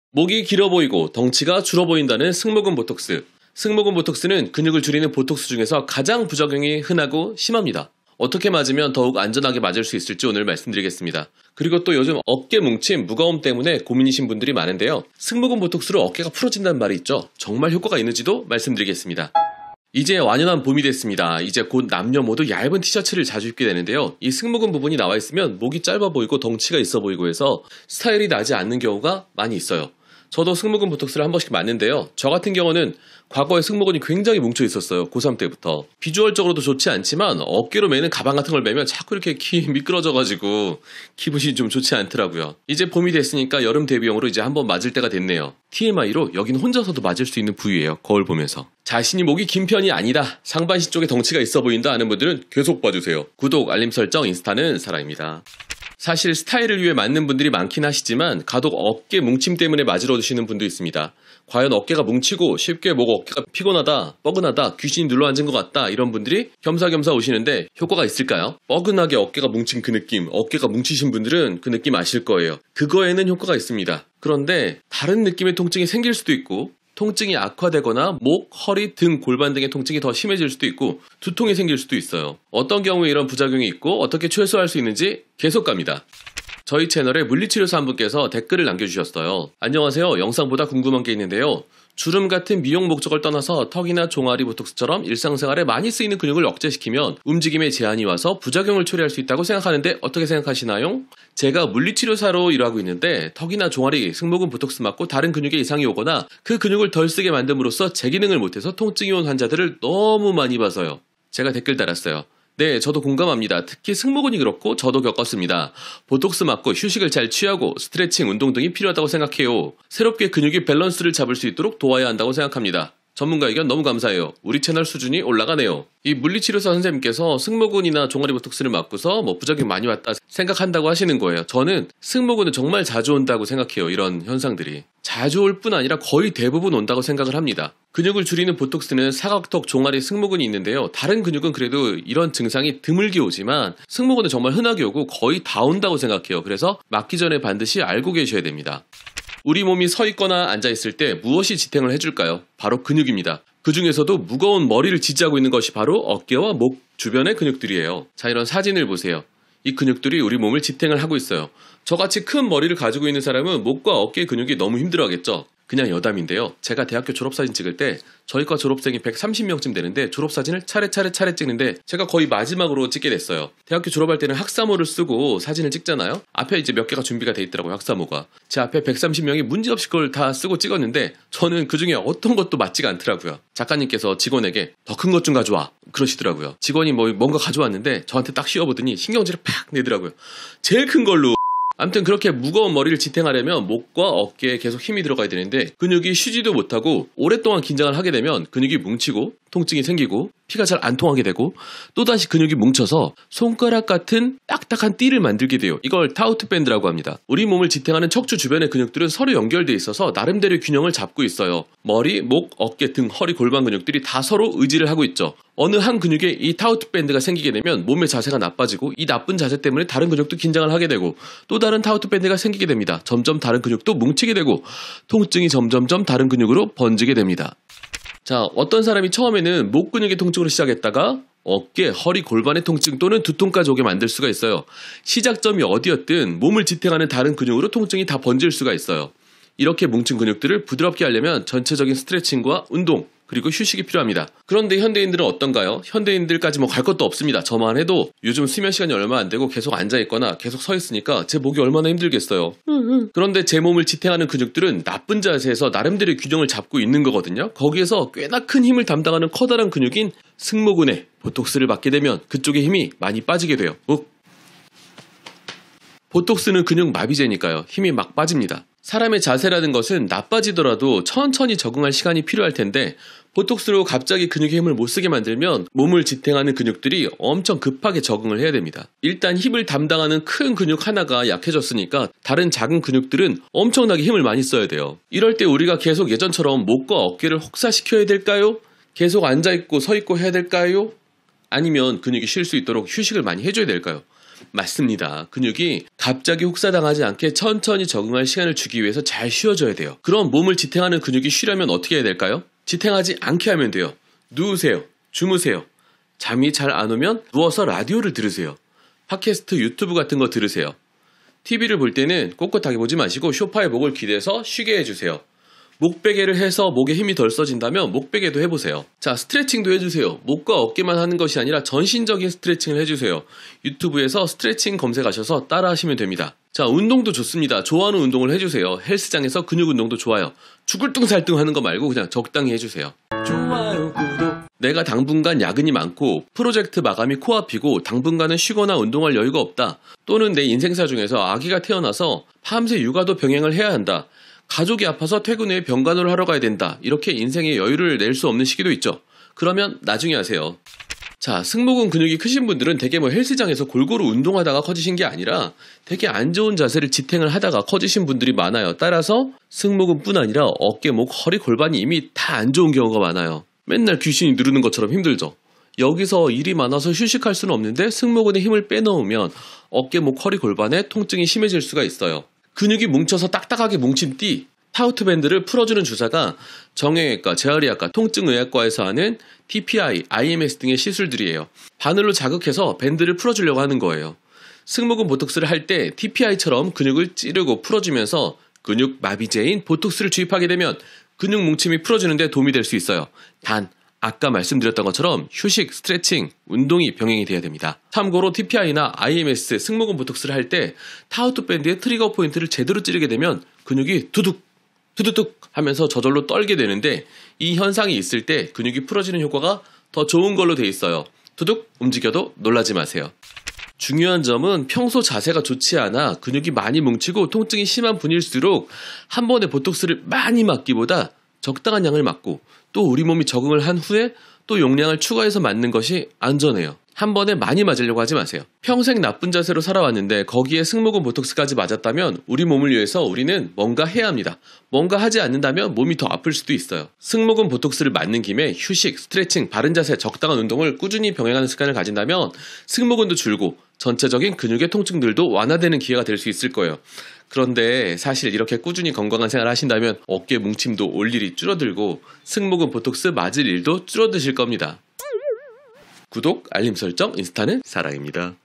승모근 보톡스 부작용은 거의 모두 겪습니다. 꼭 하기전에 보세요 효과, 부작용.
승모근 보톡스 부작용은 거의 모두 겪습니다. 꼭 하기전에 보세요